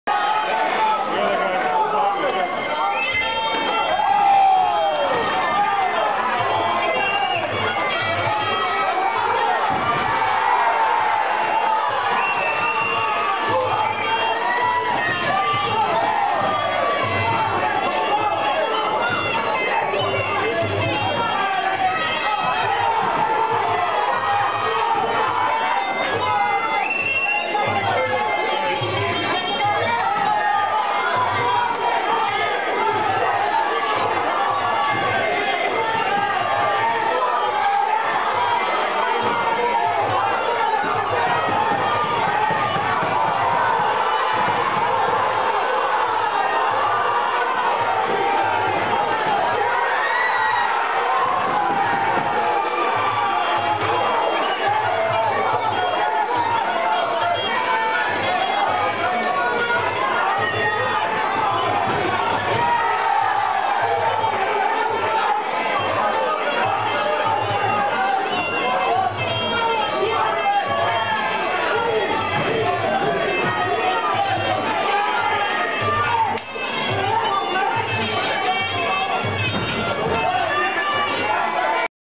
Muay Thai Anthony killeen v Ray Douglas Part 1
07 Nov 2010 Loughrea Spa hotel Galway Ireland